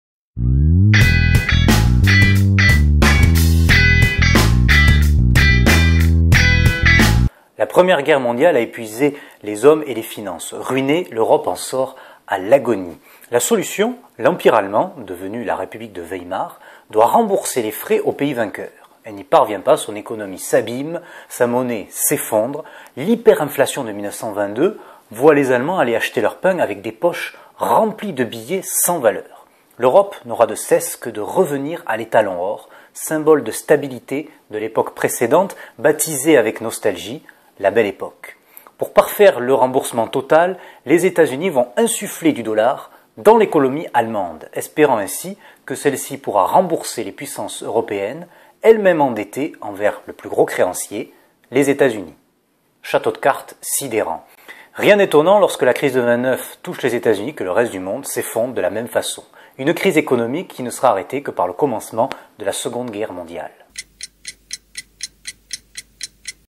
La minute - Rembourser la Guerre
La Première Guerre mondiale a laissé l'Europe à l'agonie. Pour s'en sortir, les pays vainqueurs demandent réparation à la jeune république de Weimar.
La Minute, c'est une émission chronométrée pour un aperçu d'un événement historique !

→ Pour aller plus loin :

*Droz Jacques, Que sais-je ? : Histoire de l'Allemagne, Chapitre V : La République de  Weimar, PUF

*Keynes J.M.; Bainville J., Les conséquences économiques de la Paix, Les conséquences politiques de la Paix, TEL, Gallimard